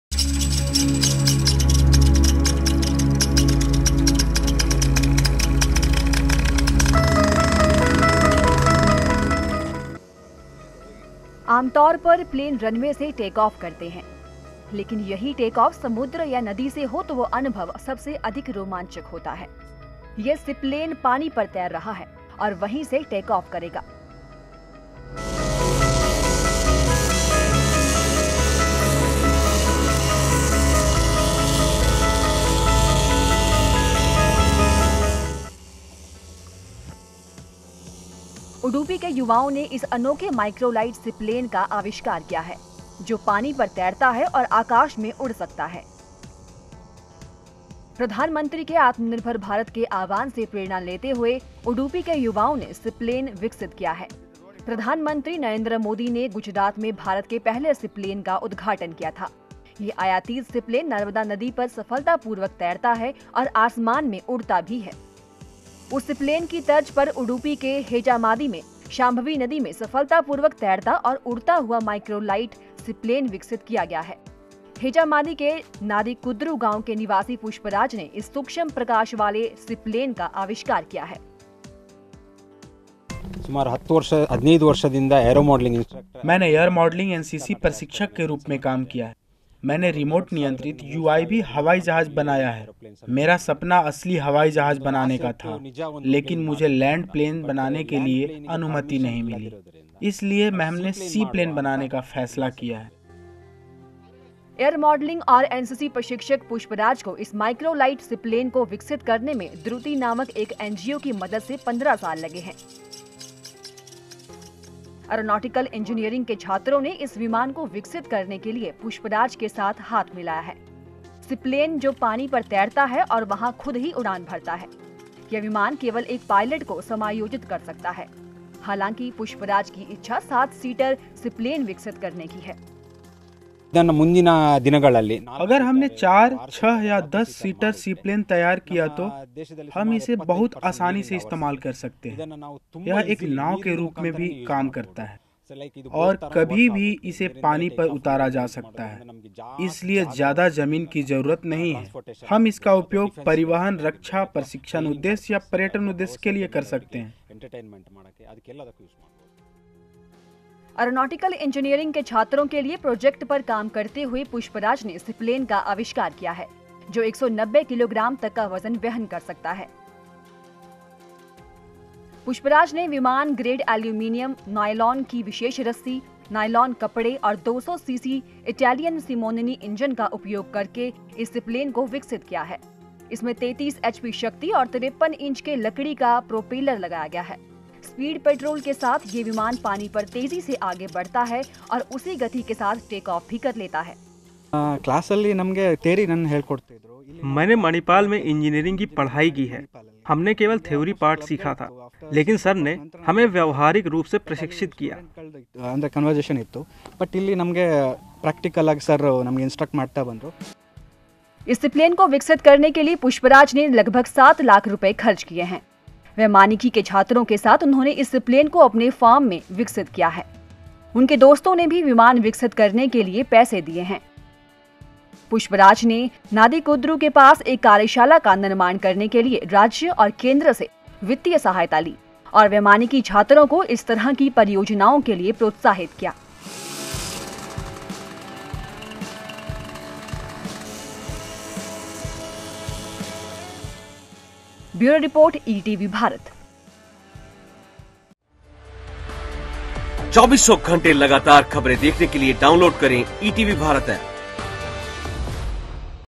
Seaplane which floats on water and flies in the sky | Micro-light Seaplane | ETV Bharat Delhi
आमतौर पर प्लेन रनवे से टेक ऑफ करते हैं लेकिन यही टेक ऑफ समुद्र या नदी से हो तो वह अनुभव सबसे अधिक रोमांचक होता है.यह सी प्लेन पानी पर तैर रहा है और वहीं से टेक ऑफ करेगा. उडुपी के युवाओं ने इस अनोखे माइक्रो-लाइट सी प्लेन का आविष्कार किया है जो पानी पर तैरता है और आकाश में उड़ सकता है. प्रधानमंत्री के आत्मनिर्भर भारत के आह्वान से प्रेरणा लेते हुए उडुपी के युवाओं ने सीप्लेन विकसित किया है.